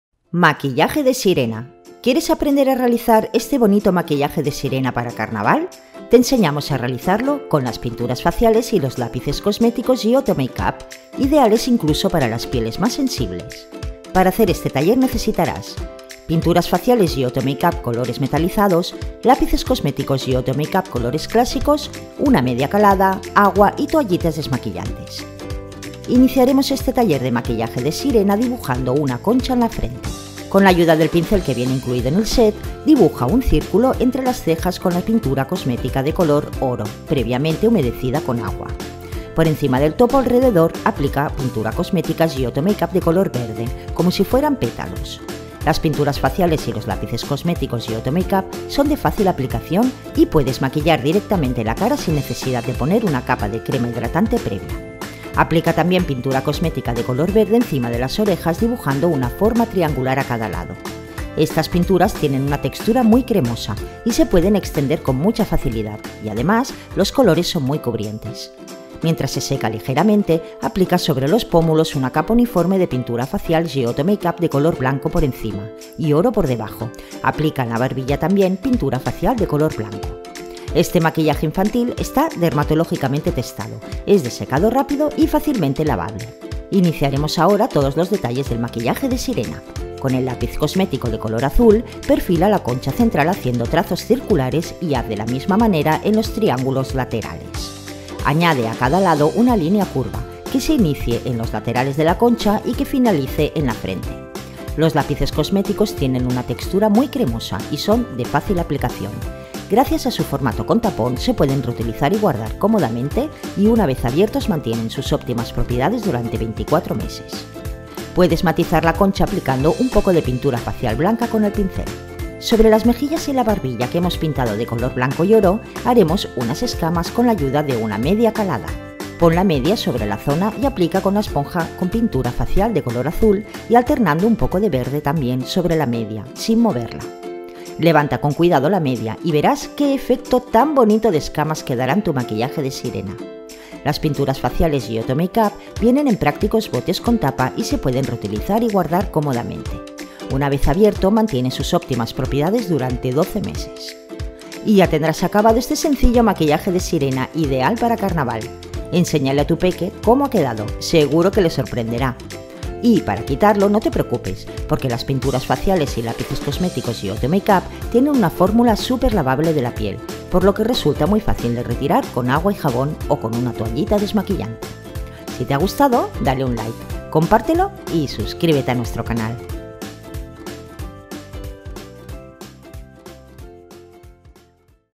Maquíllate como una bonita Sirena| Maquillajes para niños
¿Te gustaria aprender a realizar este bonito maquillaje de sirena para Carnaval? 🧜‍♀️Te enseñamos a realizarlo con ¡las pinturas faciales y los lápices cosméticos Giotto Make Up, ideales incluso para las pieles más sensibles! 😄

Para hacer este maquillaje necesitarás:
- Pinturas faciales Giotto Make Up colores metalizados
- Lápices cosméticos Giotto Make Up colores clásicos [F474200]
- Media calada
- Agua
- Toallitas desmaquillantes

1. Con la ayuda del pincel dibuja un círculo entre las cejas con la pintura cosmética de color oro. Por encima del círculo, aplica la pintura de color verde como si fueran pétalos y dibuja encima de las cejas una forma triangular a cada lado. 🌸

2. Aplica con la esponja de manera uniforme una capa de pintura blanca en los pómulos y en la barbilla. Con el lápiz cosmético de color azul, perfila la concha central, previamente hecha en la frente. 🐚

3. Para terminar, en las mejillas, aplica con la ayuda de la esponja un poco de pintura facial de color azul y verde encima de la media  y, una vez aplicada, levántala para ver el efecto de escamas. 😄

Y ¡ ya tendrás tu maquillaje de sirena! 🧜‍♀️

¿Te animas? 🥰